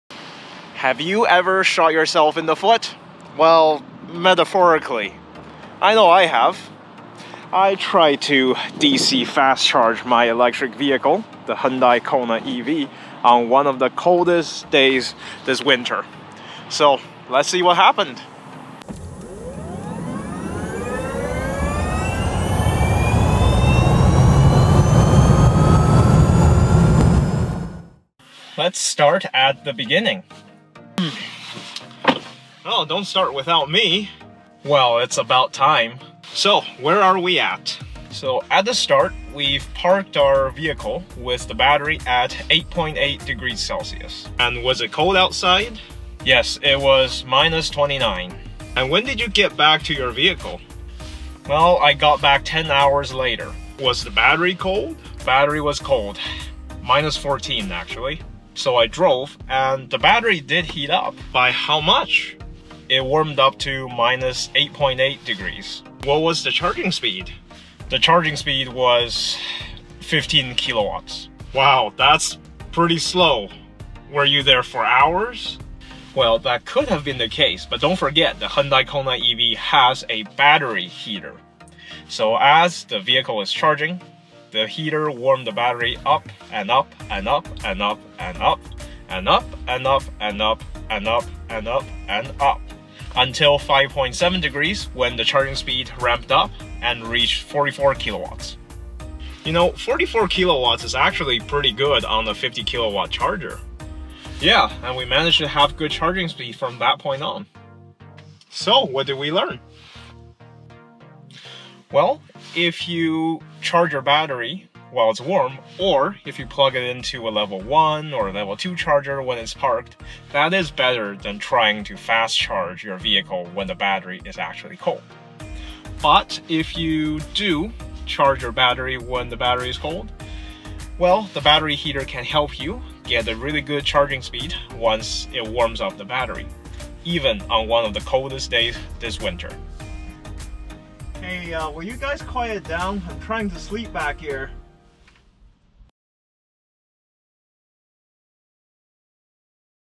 Hyundai Kona EV Winter Charging: Level 3 Charging with a Very Cold Battery
Electric Vehicle in the cold is what everyone worries about. This a quick overview of a charging session in the extreme Canadian winter.
Ambient Temperature is -26°C
Hyundai Kona EV's battery at the start of the charging session is -8.8°C
Hyundai Kona EV's initial charging speed is 15 kW
Hyundai Kona EV's final charging speed is 44 kW
Hyundai Kona EV reached 44kW when the battery reached 5.7°C

Charging was done at the 50kW Flo Charger in Londonderry Mall, Edmonton, Alberta

A more detailed video about this charging session is coming and there are lots of valuable information about EV charging to be uncovered!